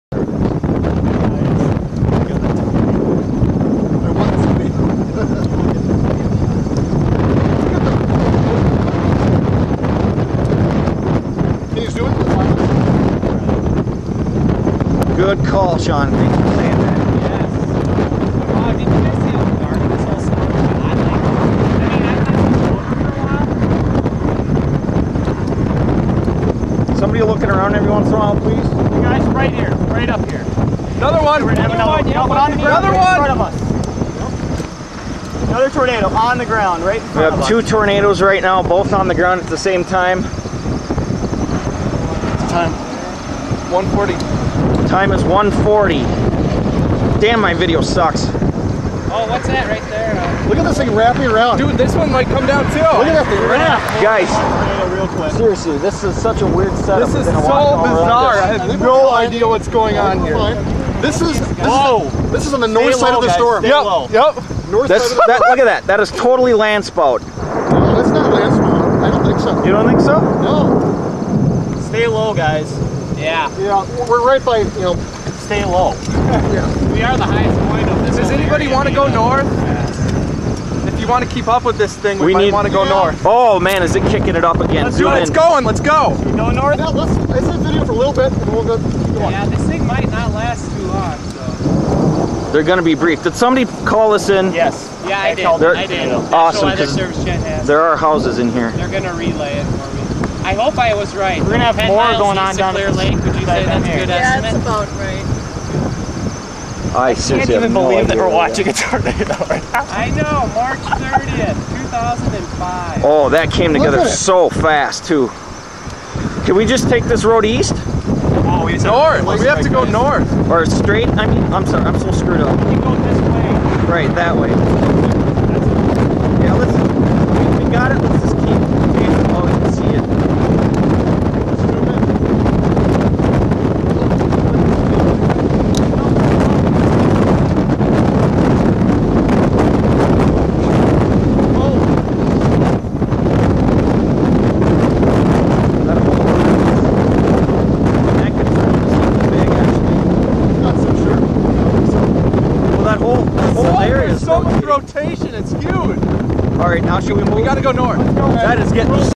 Cold Core Tornadoes Strike Iowa
Unusual cold core tornadoes strike Iowa. These landspout-like tornadoes are taken by storm chasers.